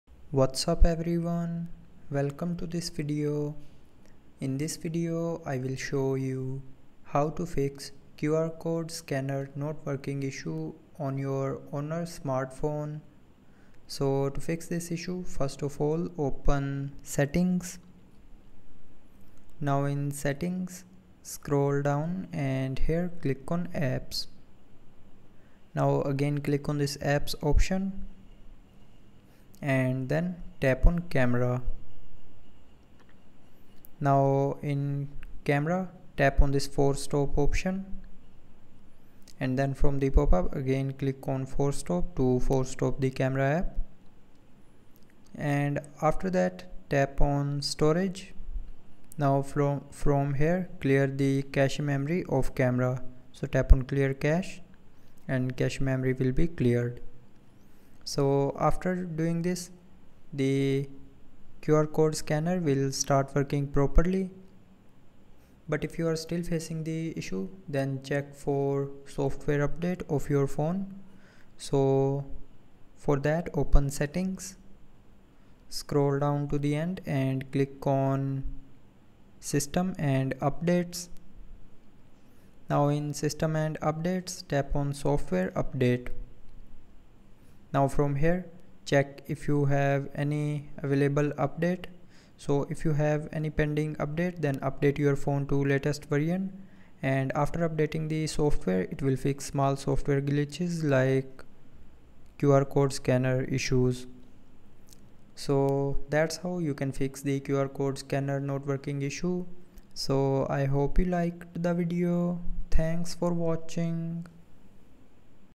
How to Fix Honor Phone QR Code Scanner Not Working
in this video you will learn How to Fix Honor Phone QR Code Scanner Not Working